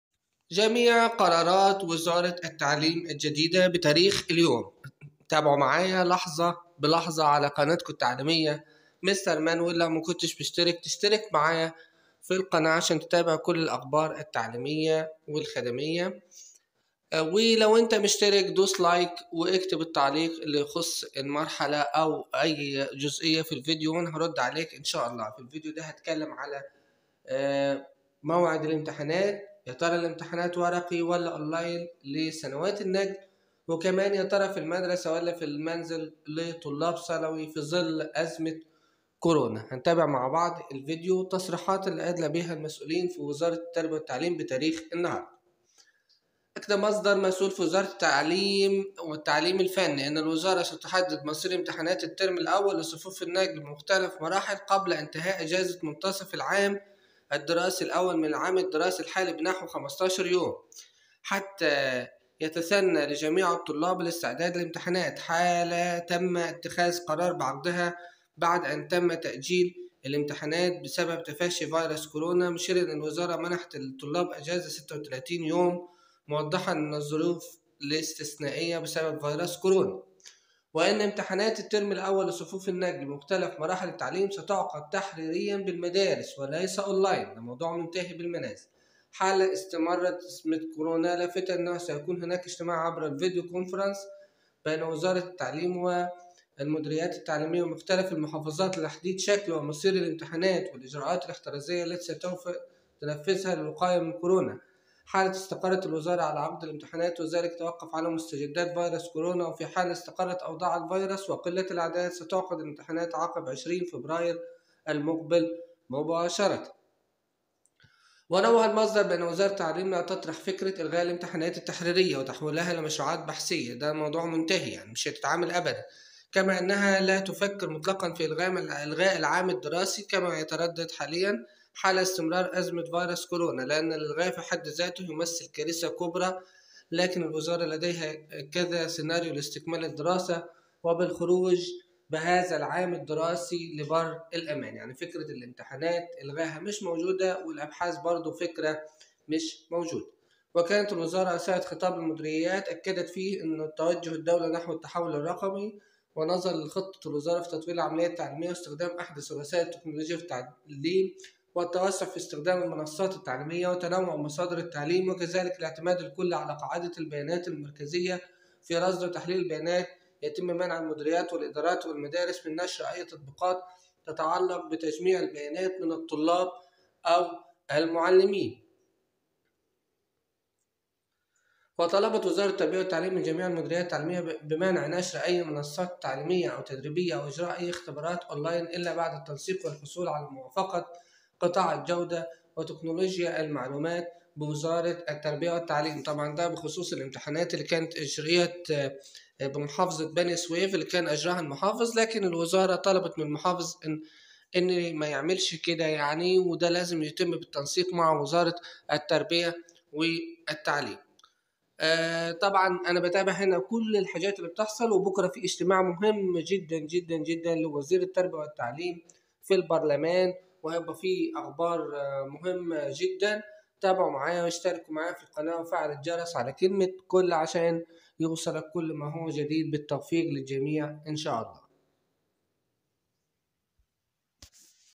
(موعد الامتحانات/ورقي ولا اون لاين /في المدرسة ولا المنزل) وزارة التعليم ترد في بيان رسمي
لا نية لتأجيل الامتحانات
الامتحانات في موعدها
لا وجود الامتحانات الاون لاين لسنوات النقل
لا وجود الامتحانات اون لاين للشهادة الاعدادية

س ـ ما مصير الفصل الدراسى الأول فى ضوؤ القرارات الأخيرة؟
ج ـ توقف الدراسة بداية من 2 يناير لن يذهب أى طالب للمدرسة.

س ـ هل ينطبق القرار على جميع المدارس ؟
ج ـ يشمل جميع نوعيات المدارس دولى وخاص وحكومى وجامعات.

س ـ هل سيتم حذف المناهج ؟
ج ـ المناهج سيتم استكمالها بنظام التعليم عن بعد.

س ـ كم المدة المتبقية من الترم الأول؟
ج ـ الترم الأول مستمر الدراسة أون لاين من 2 حتى 15 يناير المقبل.

س ـ هل مطلوب ذهاب الطلاب للمدارس؟
ج ـ منع ذهاب الطلاب للمدارس سيكون الأفضل حرصا على سلامتهم.

س ـ متى تبدأ إجازة نصف العام؟
ج ـ تبدأ إجازة نصف العام 16 يناير وتستمر حتى 20 فبراير المقبل.

س ـ ما مصير امتحانات الفصل الدراسى الأول؟
ج ـ  تم تأجيلها إلى ما بعد 20 فبراير المقبل حرصا على سلامة الطلاب والمعلمين.

س ـ كيف يستكمل الطلاب المناهج الدراسية؟
ج ـ يتم استكمال المناهج من خلال القنوات التعليمية والمنصات الإلكترونية.

س ـ ما هى أبرز المنصات المتاحة؟
ج ـ منصة أدمودو ومنصة التعلم ببنك المعرفة ومنصة ذاكر حيث وفرت الوزارة جميع المقررات لكل الصفوف الدراسية على المنصات والقنوات التعليمية.

س ـ كيف يتم تقييم الطلاب فى الصفوف الأولى؟
ج ـ الطلاب فى الصفوف الأولى لن يتم امتحاناتهم تحريريا ولن تقييم من خلال معلم الفصل لقياس المهارات الفردية والجماعية ويرمز لها بألوان.

س ـ متى تعقد امتحانات الترم الأول للصفوف من الرابع حتى الثالث الإعدادي؟
ج ـ بعد 20 فبراير المقبل سيتم تقدير الموقف وتحديد موعد بعد الامتحانات.

س ـ ماذا لو لم يتم عقد امتحانات الفصل الدراسى الأول؟
ج ـ ستجرى امتحانات المراحل التعليمية عقب أجازة منتصف العام، حال استقرار أعداد الإصابات بكورونا أو تأجيلها للعام الدراسى الثاني.

س ـ كيف يتم عقد امتحانات الصفين الأول والثانى الثانوى؟
ج ـ إجراء امتحان المرحلة الثانوية أولى وثانية من  في المدرسة بنظام التابلت.

س ـ هل فكرة تقييم الطلاب فى صفوف النقل من خلال الأبحاث مطروحة؟
ج ـ لم يتم طرحها ولكن الحديث عن إجراء امتحانات للطلاب.

س ـ ما مصير الامتحانات التى كانت ستعقد الأسبوع المقبل للمواد غير المضافة للمجموع والمستوى الرفيع لطلاب مدارس اللغات؟
ج ـ تتوقف جميع الامتحانات ولا مجال لإجراء أى اختبارات حاليا.

س ـ ما مصير الامتحانات التى أداها الطلاب فى بعض المواد؟
ج ـ تحتفظ المدارس بتقييم الطلاب فيها لحين استكمال الامتحانات بعد 20 فبراير المقبل.